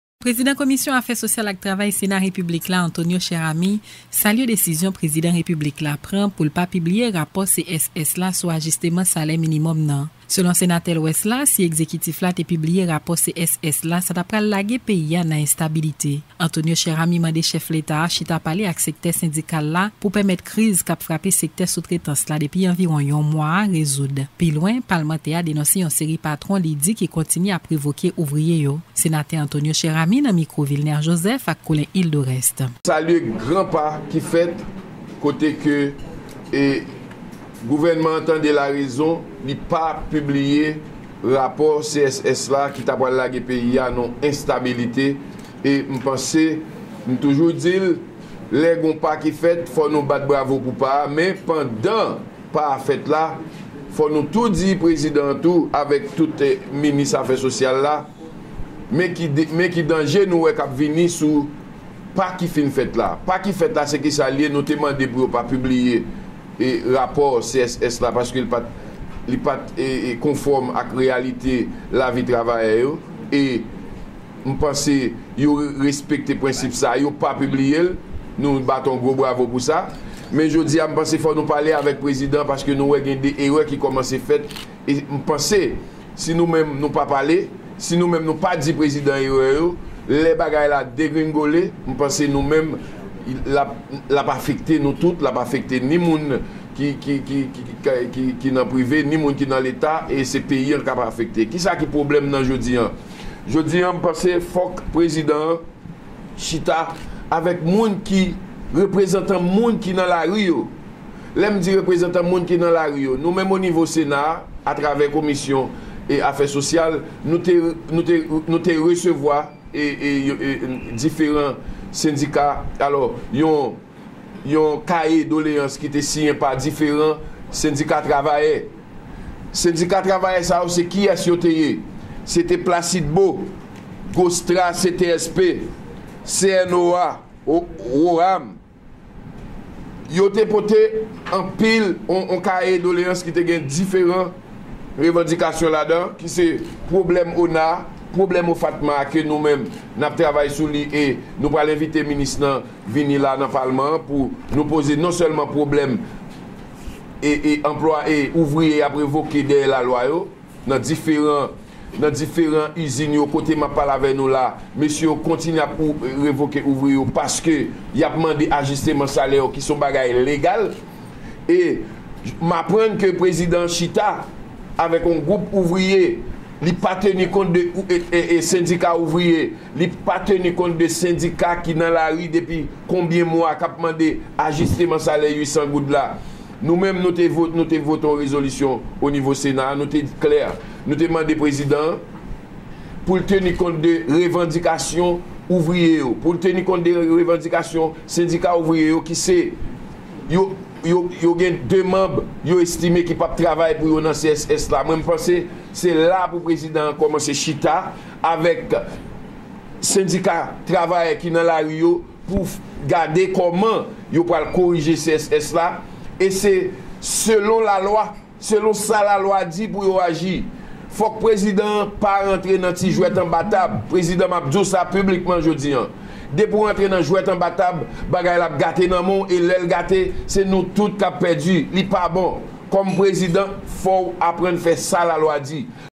Rapport du CSS: Le Sénateur Antonio Cheramy alias Don Kato salue la non-publication du rapport
Le Président de la Commission des Affaires Sociales du Sénat, Antonio Cheramy alias Don Kato, salue la décision du Président Jovenel Moïse de ne pas publier le rapport soumis par le CSS sur le salaire minimum.

Selon lui, la publication de ce  rapport pourrait créer l’instabilité dans le pays. Par ailleurs, il appelle au dialogue le chef de l’Etat, et le secteur syndical en vue d’une résolution à la crise.

Sur un autre plan, le Sénateur Antonio Cheramy alias Don Kato dénonce la poursuite de révocations des ouvriers par certains patrons.